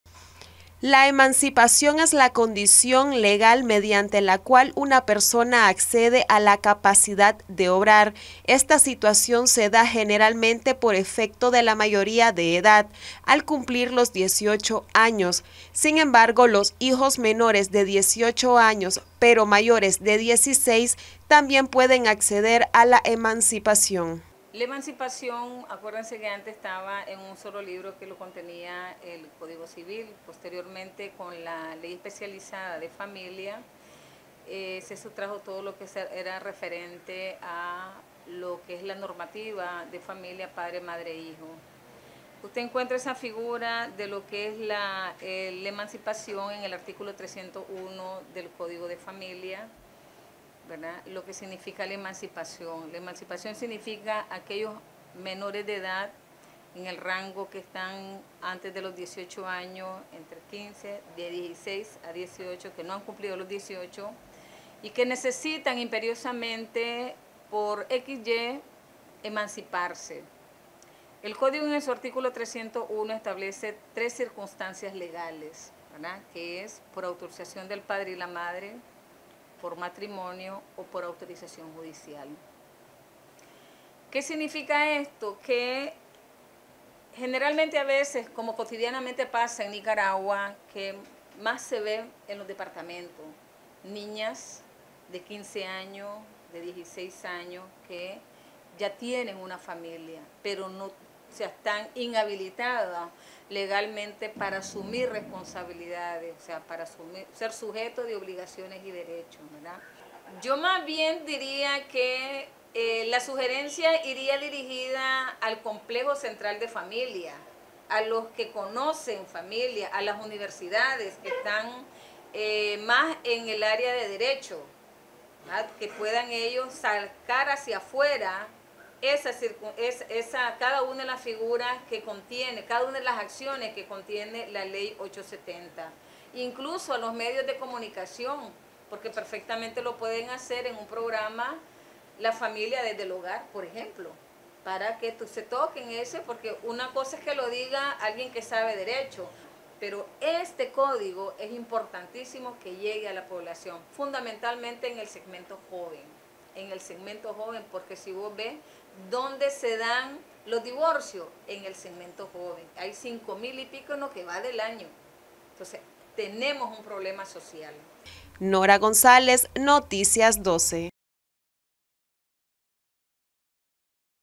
Legalidad en la emancipación de los hijos
Experta en derecho nos comenta sobre cuando se puede obtener la emancipación y que es lo que se debe de realizar en el proceso leal